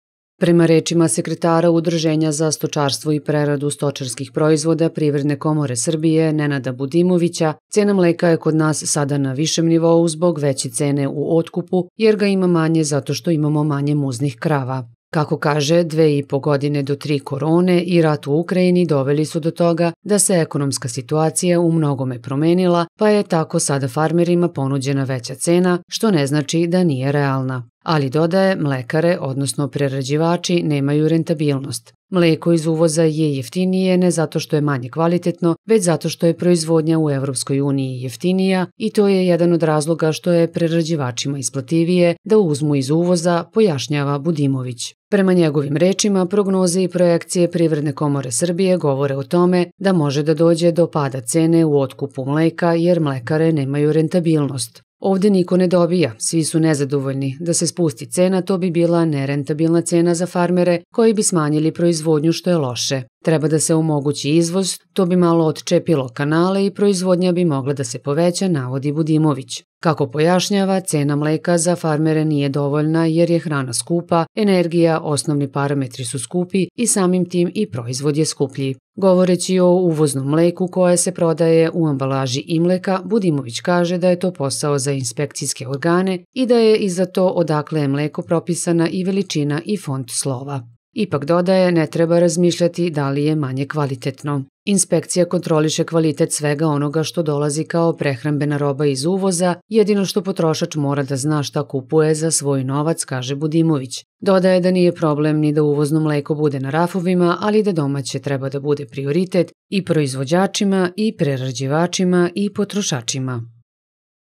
Mleko ne bi trebalo da poskupi
Sekretar Udruženja za stočarstvo i preradu stočarskih proizvoda u Privrednoj komori Srbije (PKS) Nenad Budimović smatra da mleko u prodavnicama neće biti skuplje jer je, kako kaže, stabilizovano tržište.